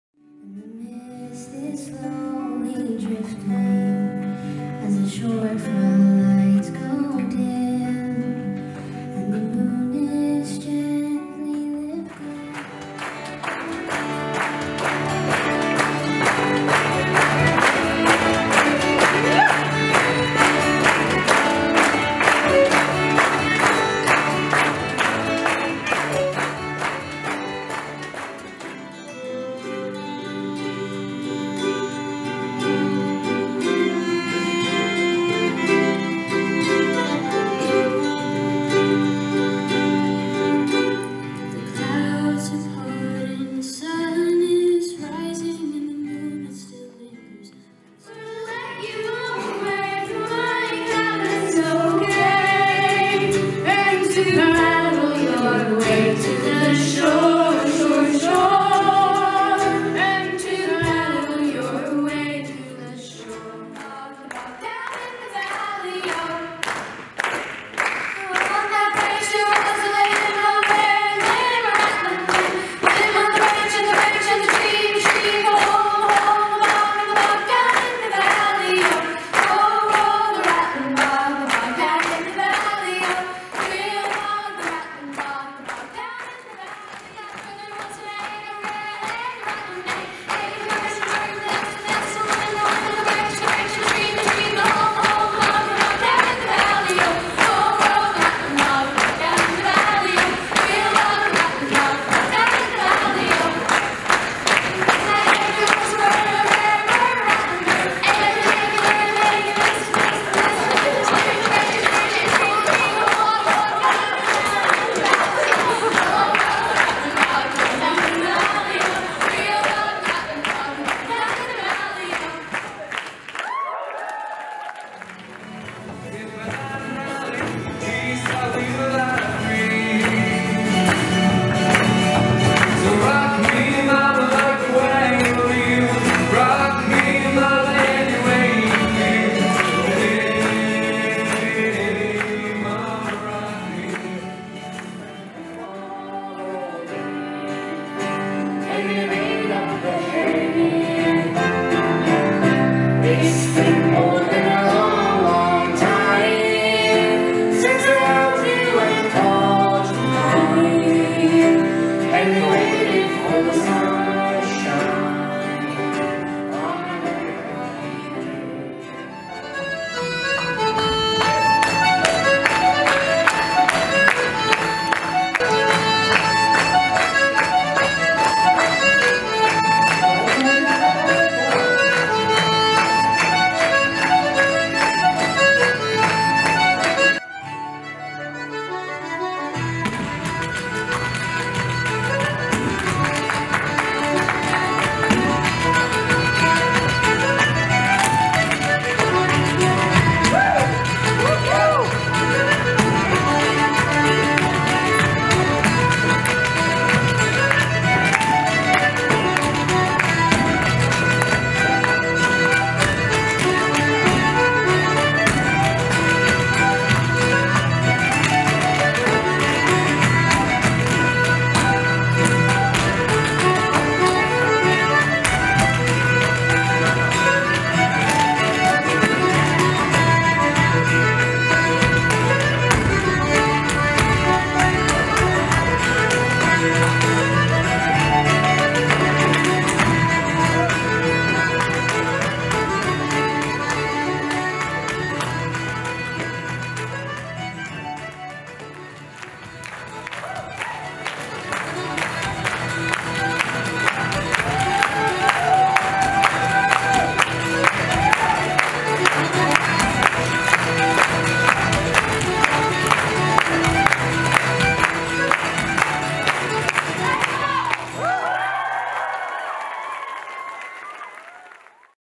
Young Folks at the Hall concert montage 2022
Young Folks at the Hall groups performing at the Lantern Centre in St. John's.  A annual folk event promoting and culturing young folk musicians in St. John's pioneered by John Clarke of NL Folk Arts Society  and NL Musician Fergus O'Bynre for 20 years. The peformance was prepared and mentored by local folk musicians after a brief weekend workshop the week before their performance.  Thank you.